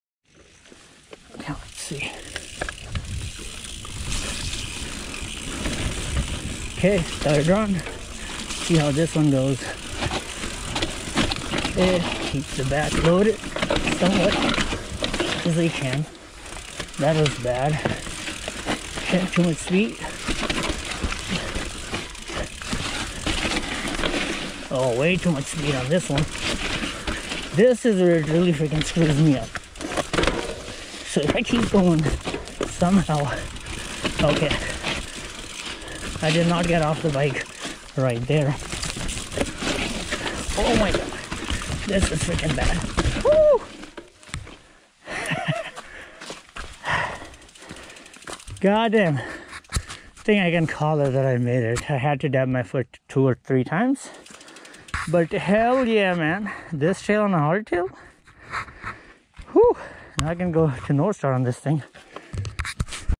CASCADE(double black) on a HARDTAIL
Trying to make it down cascade on the hardtail, and the trail is as wet as it gets lol.